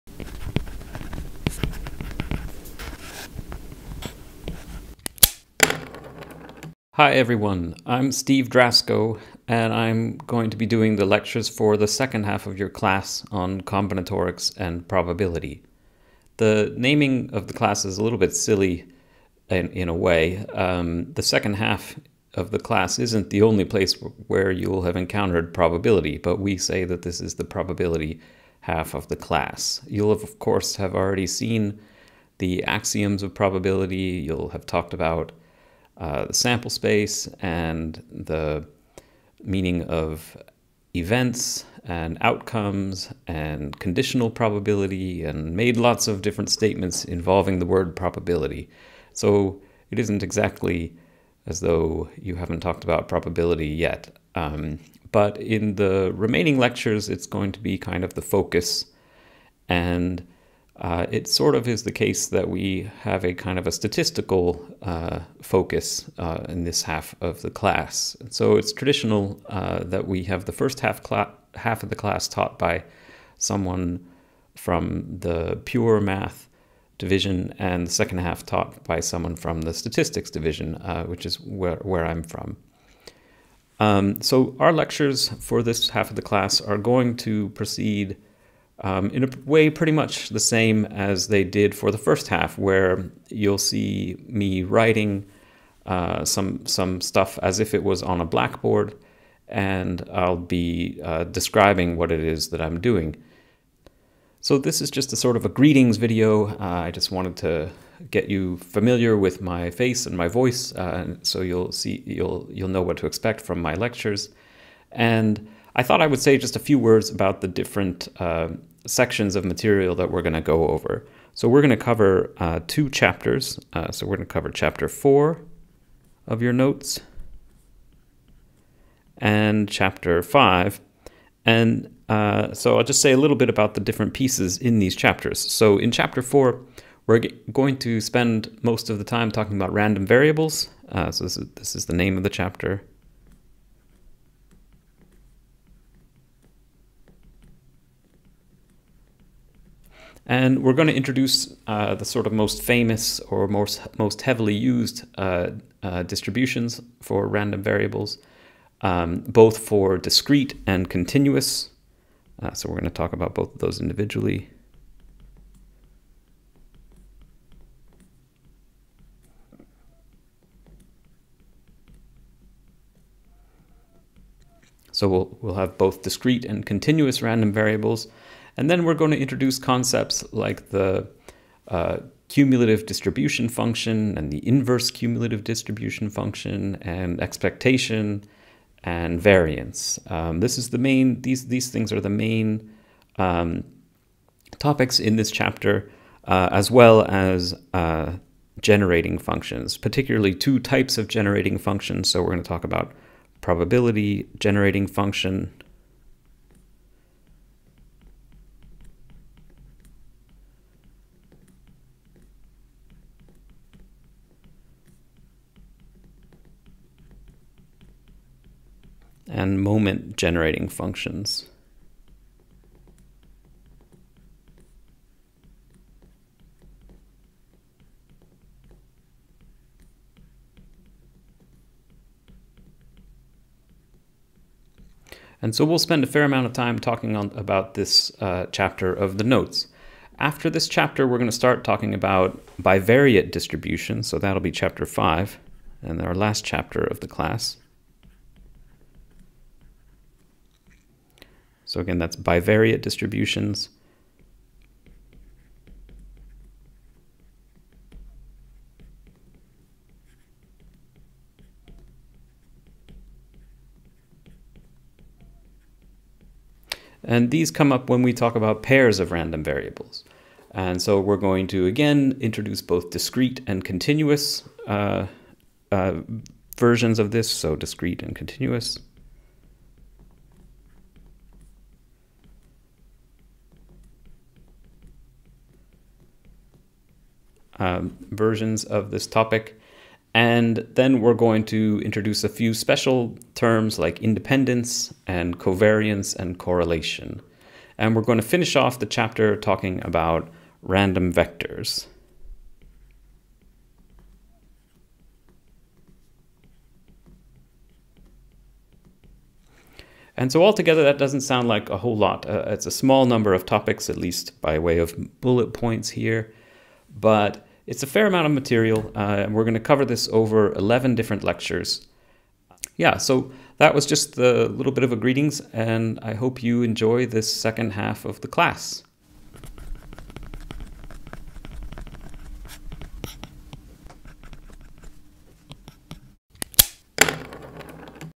Welcome to lectures on probability and random variables
A short hello and survey of the second half of a class on combinatorics and probability.

This lecture was given in 2021 as part of a Combinatorics and Probability module (MT2504) offered to undergraduate students working toward a variety of degree programs related to statistics or mathematics at the University of St Andrews, in Scotland.